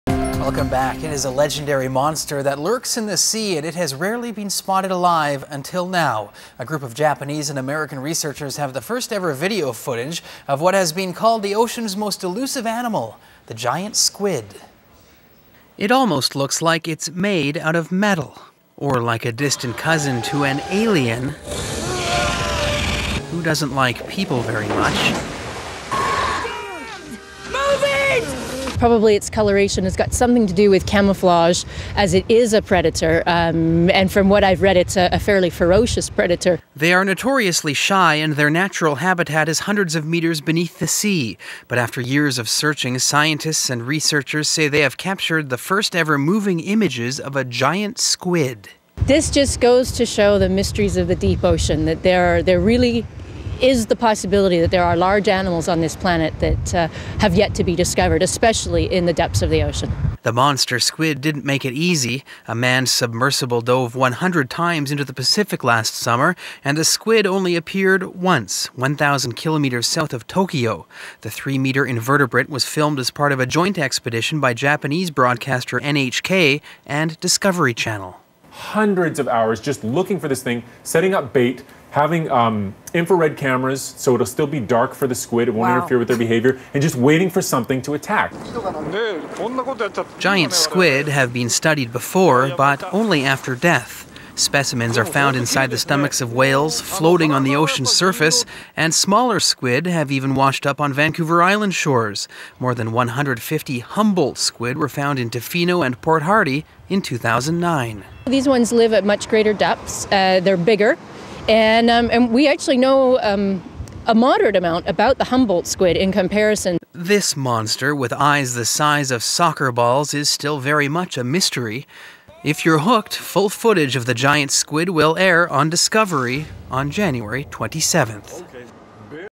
Giant Squid wows researchers
VICTORIA - It is a legendary monster that lurks in the sea. And it has rarely been spotted alive, until now.

A group of Japanese and American researchers have the first-ever video footage of what has been called the ocean's most elusive animal; the Giant Squid.